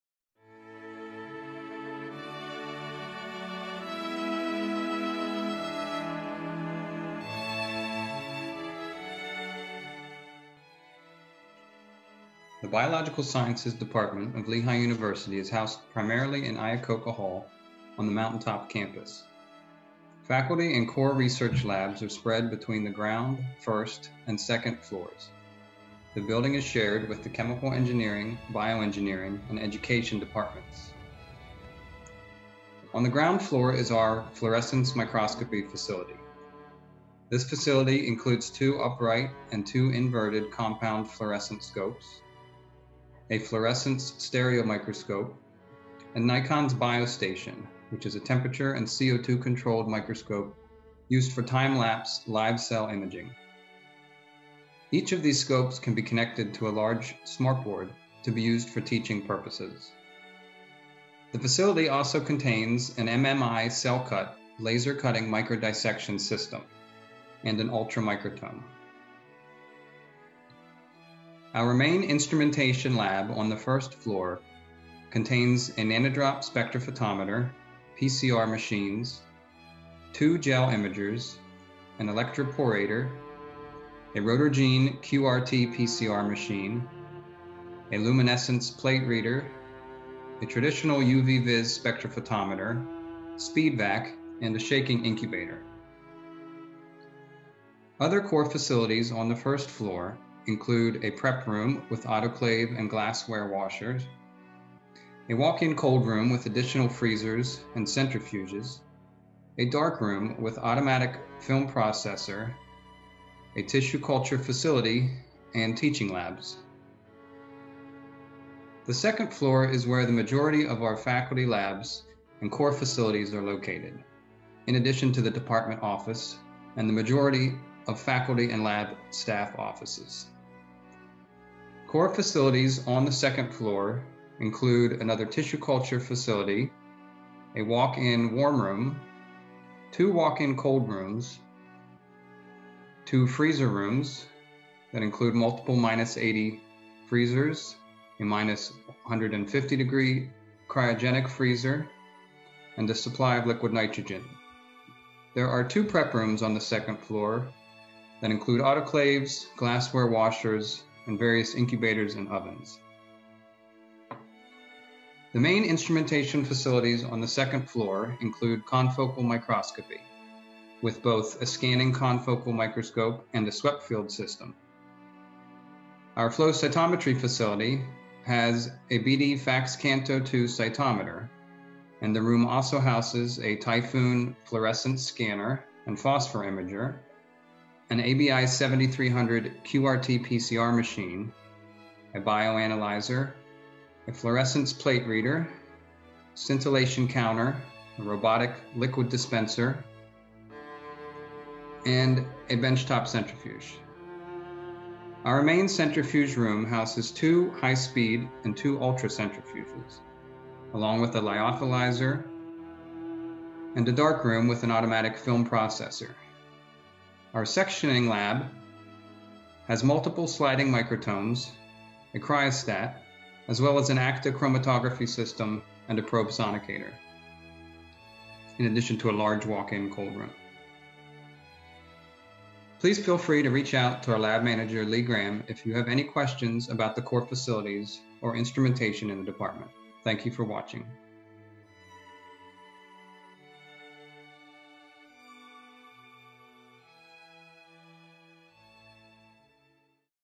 A Tour of Biological Sciences
Lee Graham, Lab Manager for the Department of Biological Sciences, takes the viewer on a tour of the department facilities in Iacocca Hall at Lehigh University, and includes an overview of scientific equipment available.